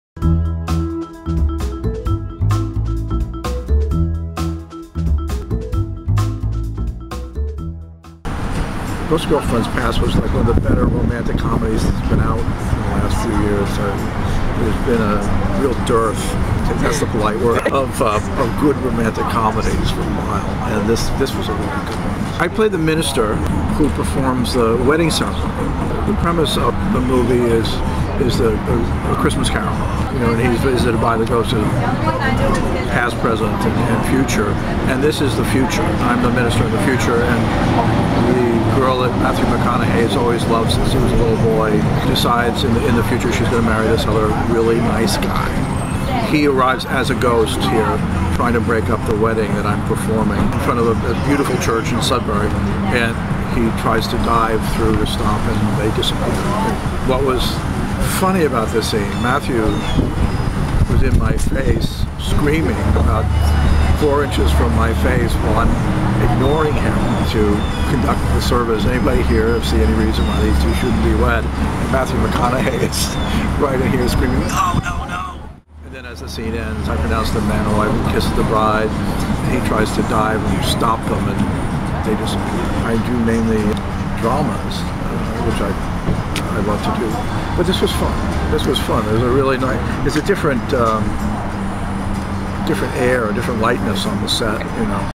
Tom Kemp Talks about his Role in "Ghosts of Girlfriends Past"
Tom Kemp, an actor who lives in Milton, discusses his role in the recent romantic comedy, "Ghosts of Girlfriends Past." Kemp can be seen in Martin Scorsese's upcoming thriller/drama, "Shutter Island," scheduled to open October of this year. The Patriot Ledger is your ORIGINAL source for local news.